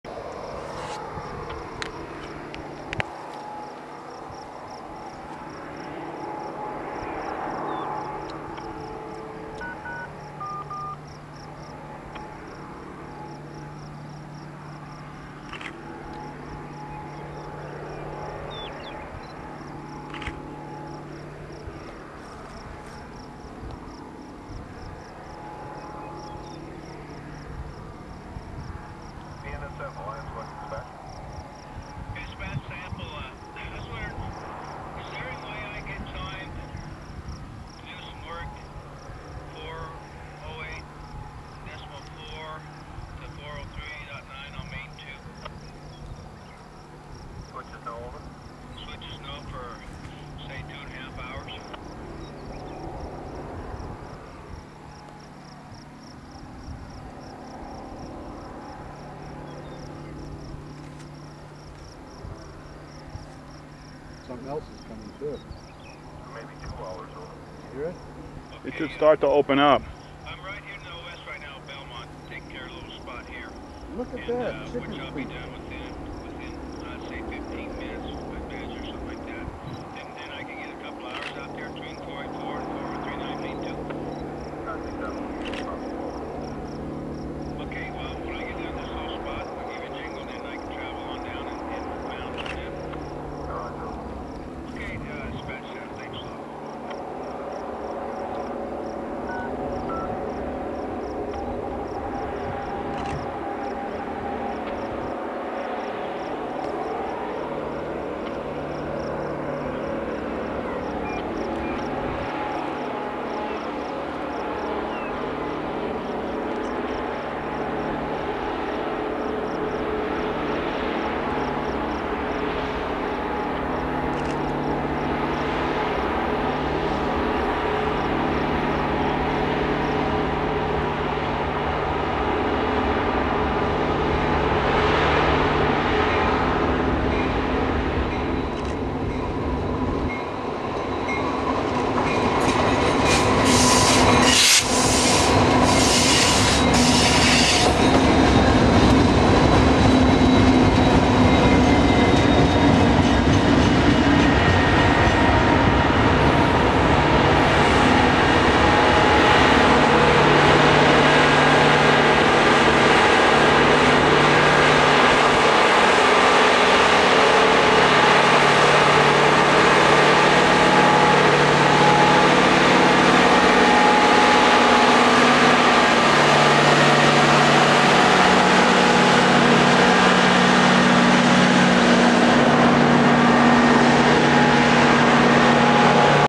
MOW and BNSF 9285
MOW equipment comes around a curve near Crawford, NE as a set of BNSF helpers led by BNSF 9285 passes it on the way back to Crawford.